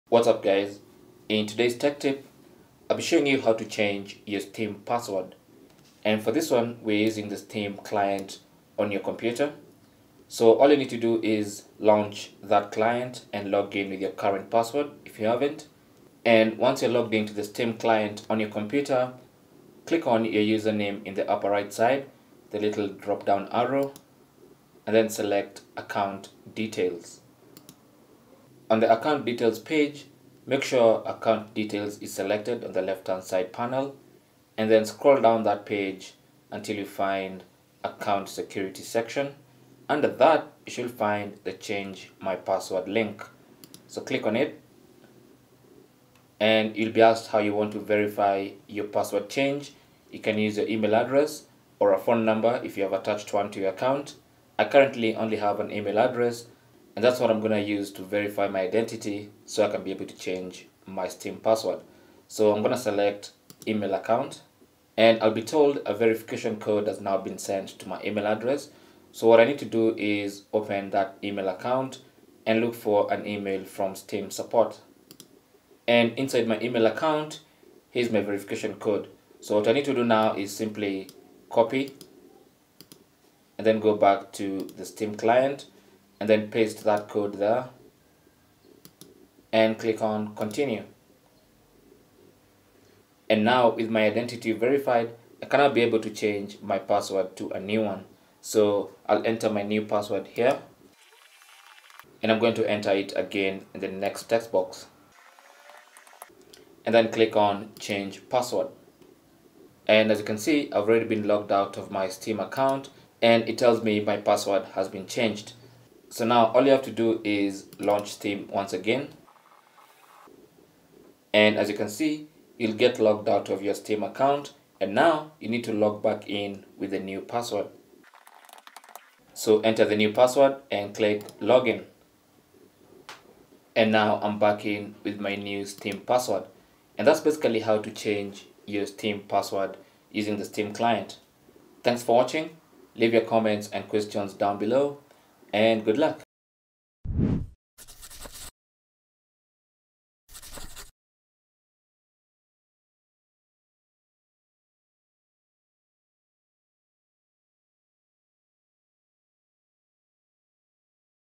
How To Change Your Steam Password
Learn how to change your Steam password in a few easy to follow steps. Changing your Steam password helps to protect your account from unauthorized access.

You can change your password on Steam client or on the website. It's the same process.

How To Change Steam Password
Go to Steam and log in if you haven't.
Click on your username in the upper right and select 'Account Details'
Click on Change Password
Verify your identity with the code sent to your Email or phone
Enter your new Steam password
Re-enter your Steam password

Timestamps:
0:00 - Intro
0:09 - How To Change Steam Password
2:00 - Try the new password
2:20 - Outro

Cell Phone Tripod Adapter